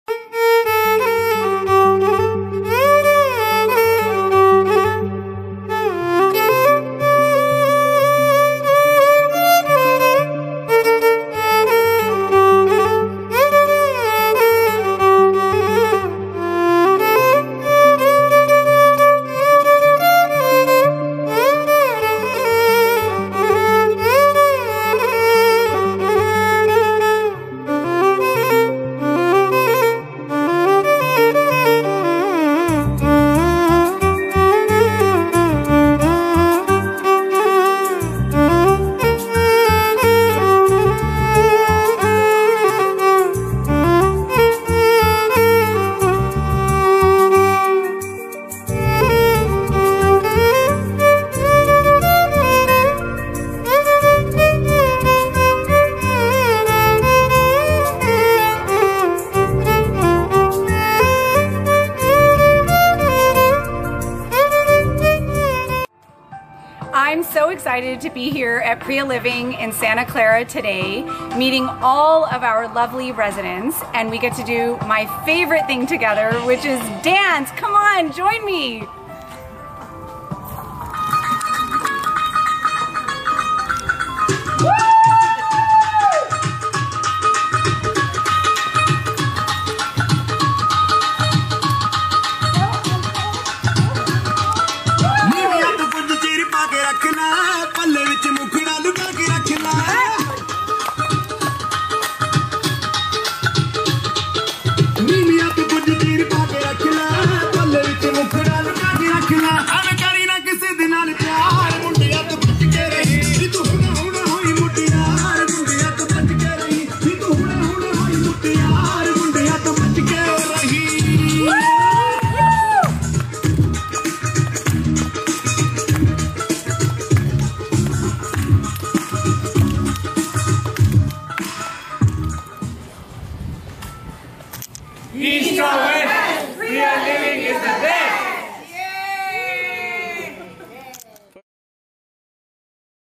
Celebrating World Senior Citizens Day at Priya Living | A Tribute on behalf of World Photography Day
🌟 Join us as we celebrate World Senior Citizens Day with a heartwarming tribute to the vibrant community at Priya Living. Through the lens of PerfectClick Photography, we capture the smiles, stories, and joy of our cherished residents. This video is a beautiful reminder of the grace, wisdom, and love that seniors bring into our lives.

Watch and be inspired by the moments that make Priya Living a truly special place. Happy World Senior Citizens Day! 💖📸